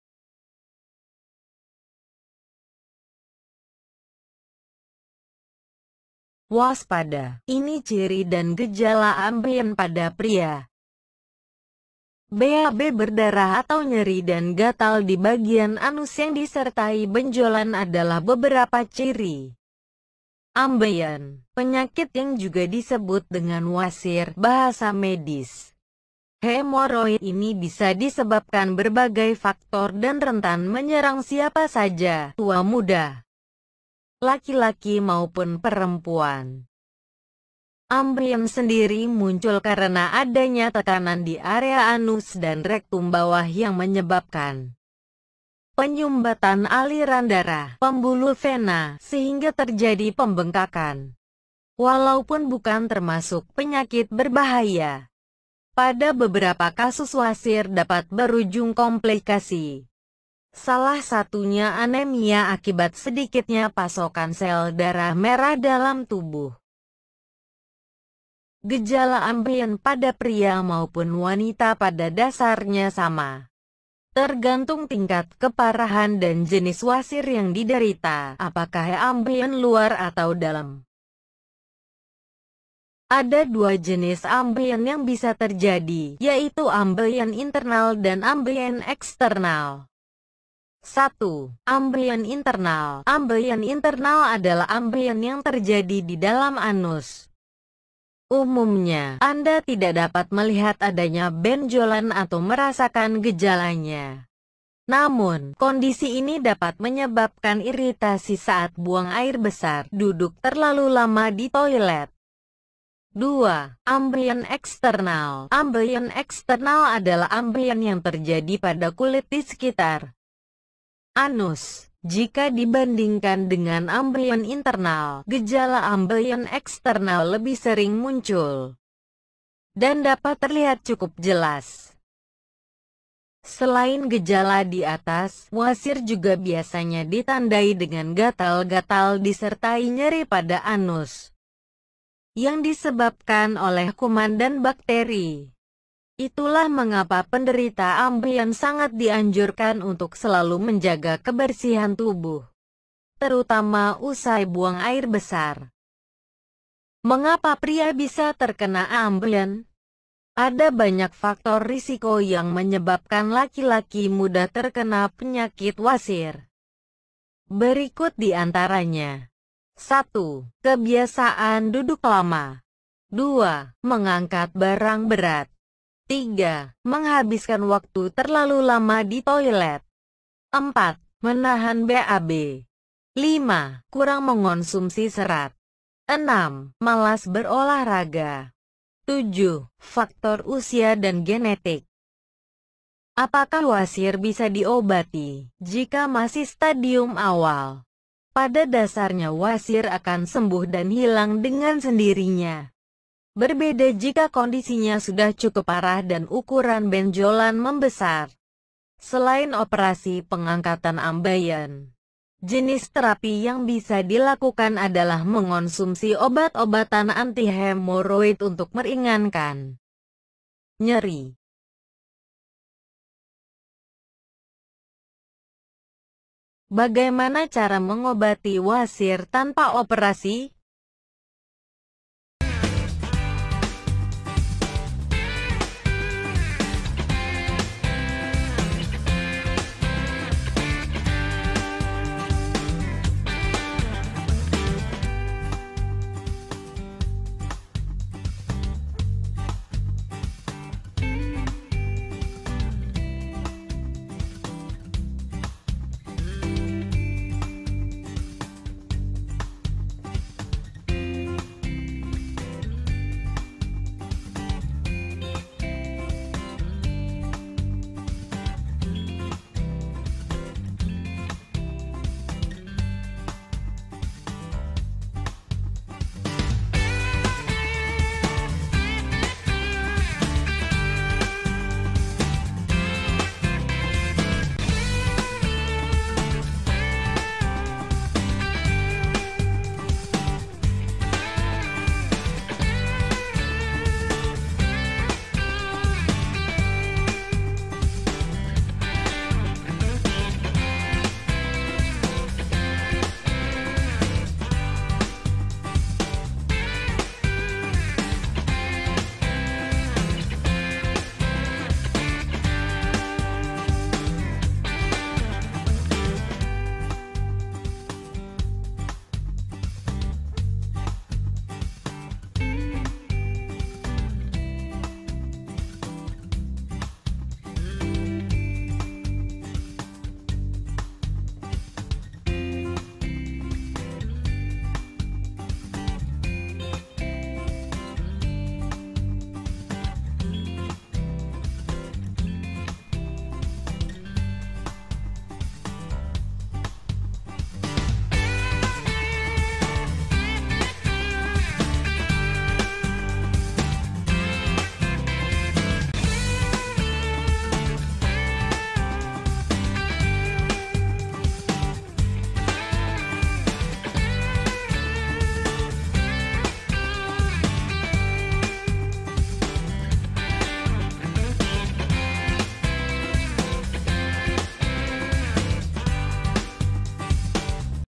Waspada! Ini Ciri dan Gejala Ambeien pada Pria
Waspada! Ini Ciri dan Gejala Ambeien pada Pria - BAB berdarah atau nyeri dan gatal di bagian anus yang disertai benjolan adalah beberapa ciri ambeien. Penyakit yang juga disebut dengan wasir (bahasa medis: hemorrhoid) ini bisa disebabkan berbagai faktor dan rentan menyerang siapa saja, tua muda, laki-laki maupun perempuan.

Ambeien sendiri muncul karena adanya tekanan di area anus dan rektum bawah yang menyebabkan penyumbatan aliran darah (pembuluh vena) sehingga terjadi pembengkakan. Walaupun bukan termasuk penyakit berbahaya, pada beberapa kasus wasir dapat berujung komplikasi, salah satunya anemia akibat sedikitnya pasokan sel darah merah dalam tubuh.

Konsultasi & Pemesanan
Kontak:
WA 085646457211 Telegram

Semoga bermanfaat.